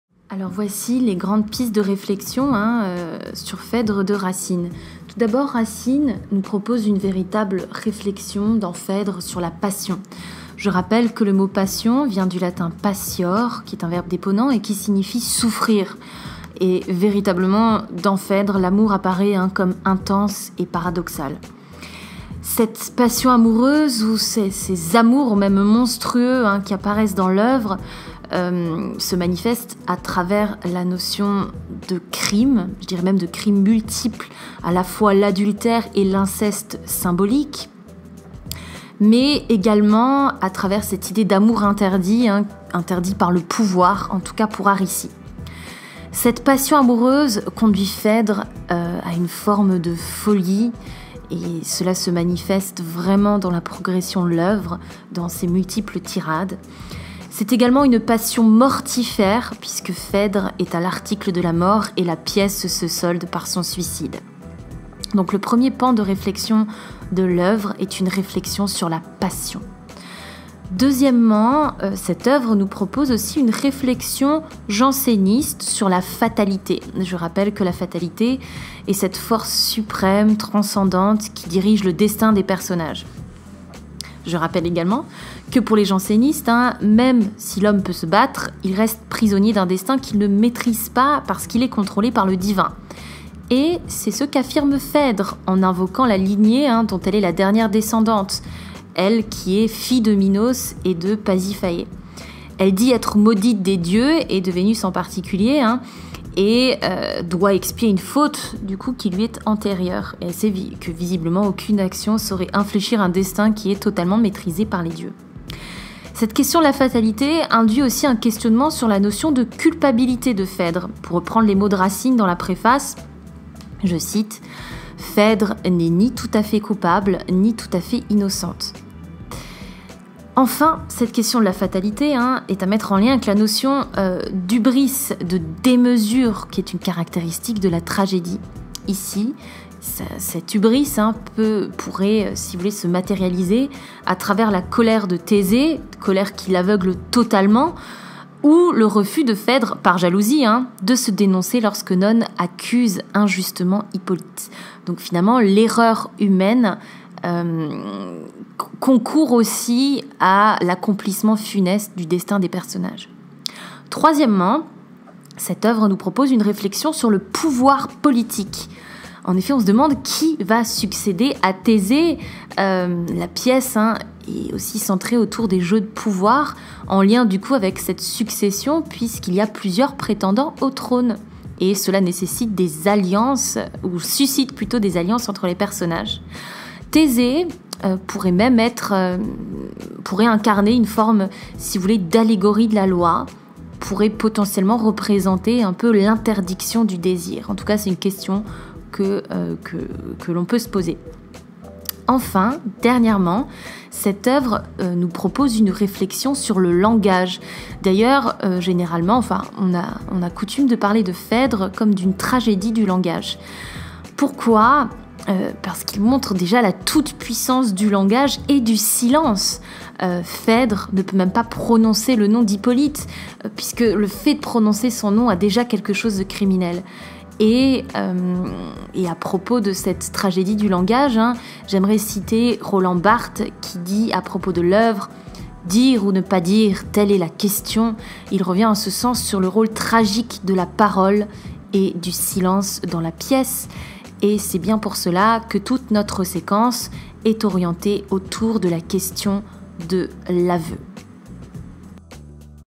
4 MINUTES POUR COMPRENDRE PHEDRE DE RACINE
Dans cette vidéo je propose un bilan de la séance 3 consacrée à la lecture de l'oeuvre. Je ne reviens pas sur l'ensemble des questions, mais simplement sur les pistes de réflexion proposées par l'oeuvre.
Consigne pour mes 1 BAC : Ecouter la vidéo, consulter le Google doc mis en ligne sur Moodle et le compléter en donnant 3 exemples de citations pour chaque thématique / piste de réflexion.